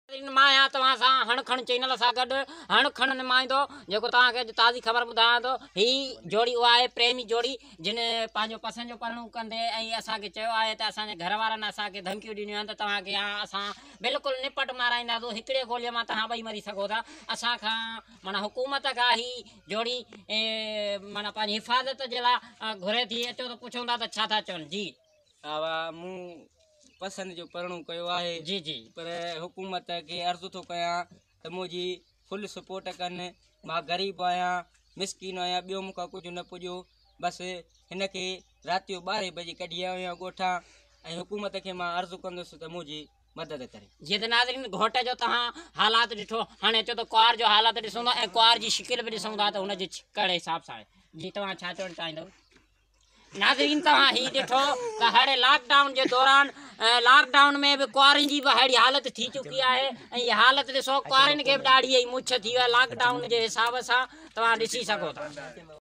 نواب شاہ پریمی جوڑ ی تحفظ کیلئے پریشان
Premi jori pareshan
Apeal in coart for protection